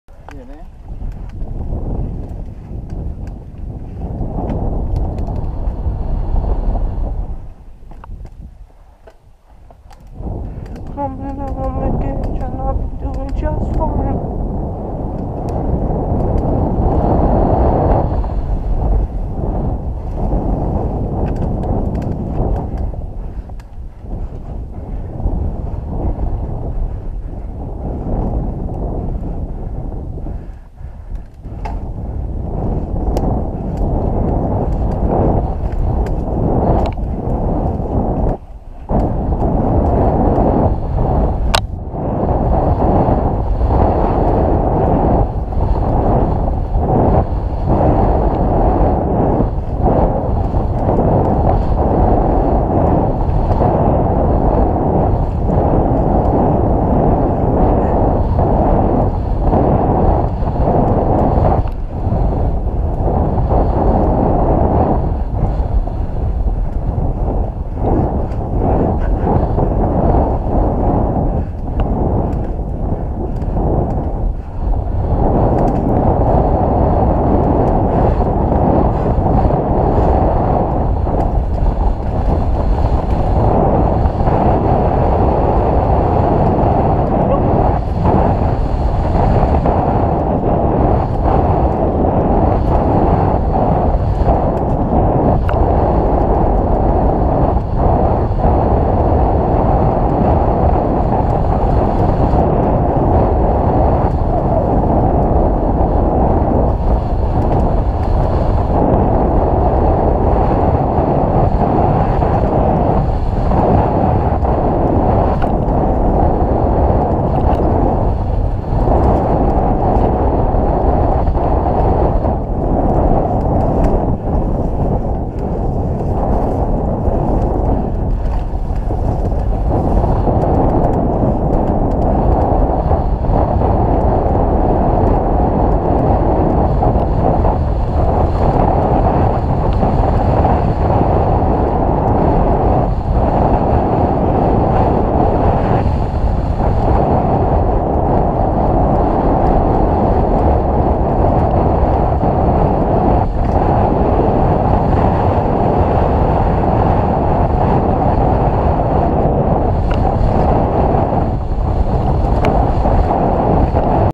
No Vacancy at Woodhill RAW SPEED
Waited five minutes for a free track just so I could send track was SUPER wet and slippy so I didn’t let out what I could’ve still my favourite track at woodhill though.